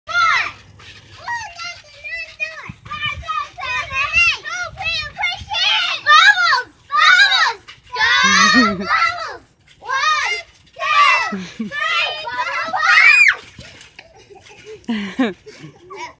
Group of girls create funny game on trampoline!
Neighborhood girls create game called "Bubbles" on trampoline.  Brother just awkwardly watches!